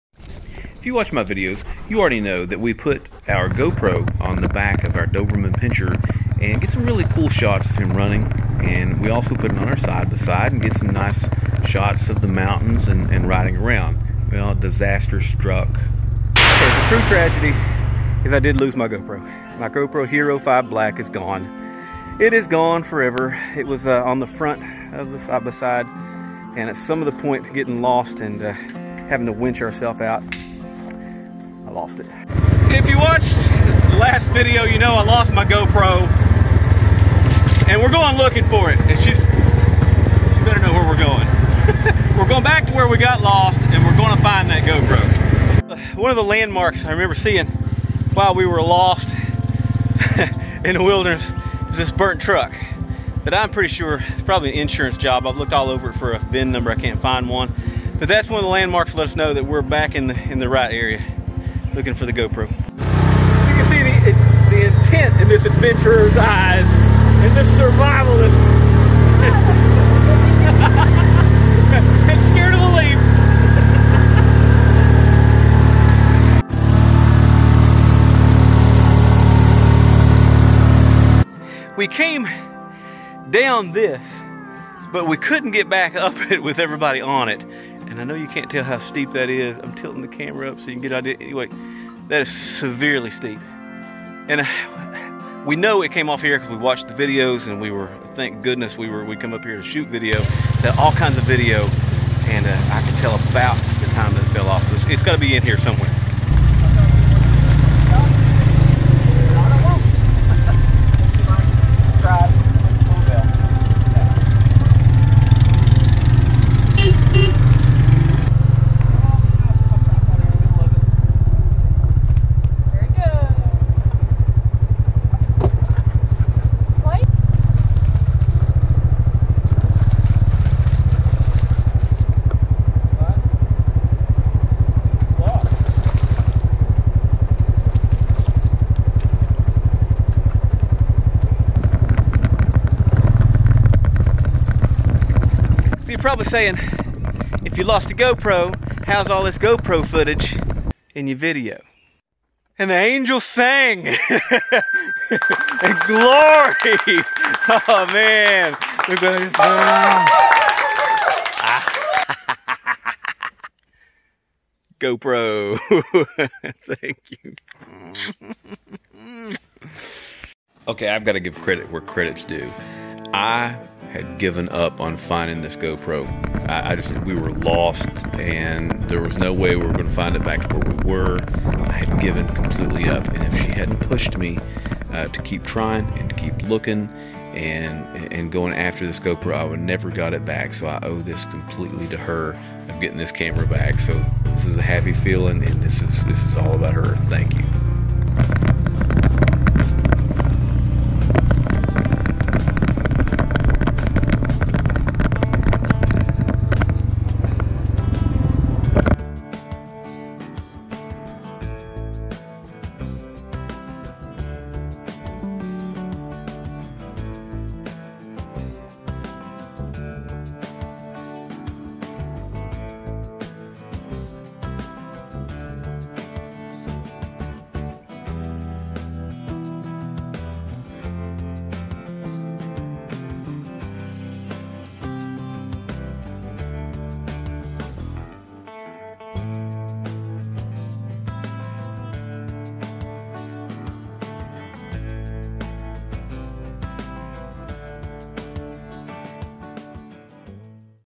LOST My GoPro Hero Camera while Exercising my Doberman Pinscher
The new GoPro Hero 5 Black is an AMAZING camera and more expensive than I could bear to lose. While taking Kruz our Doberman for a run in the WV mountains and riding our side by side we got some great footage that we feared would be lost forever.